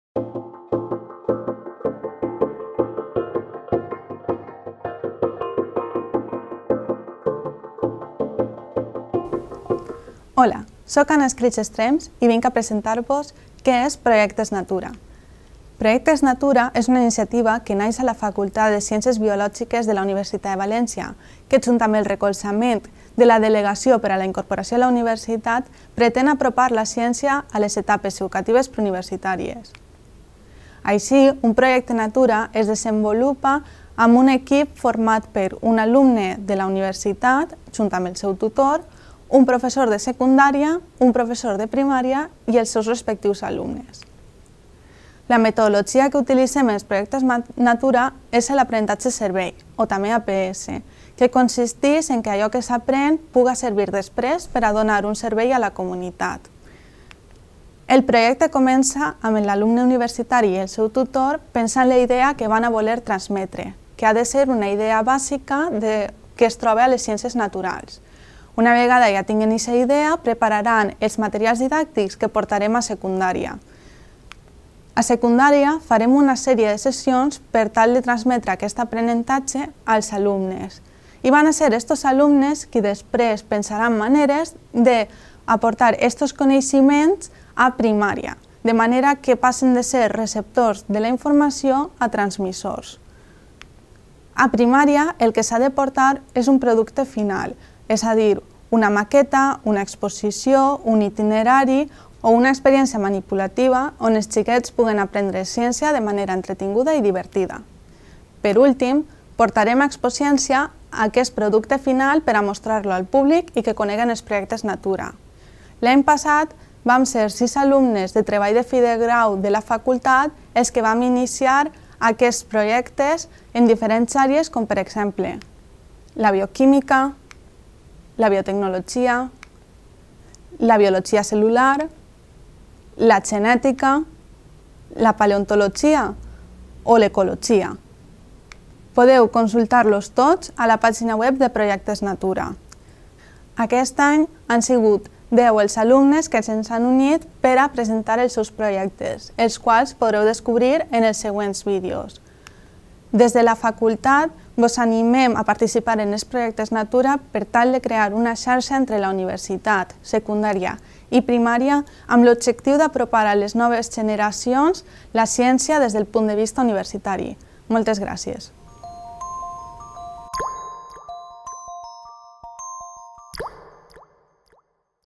Presentació Projectes Natura
Autor: Escrig, Anna
Serie: Projectes Natura
Año: 2019
Resumen: Vídeo de presentació de la iniciativa Projectes Natura que inclou una descripció dels projectes desenvolupats en el curs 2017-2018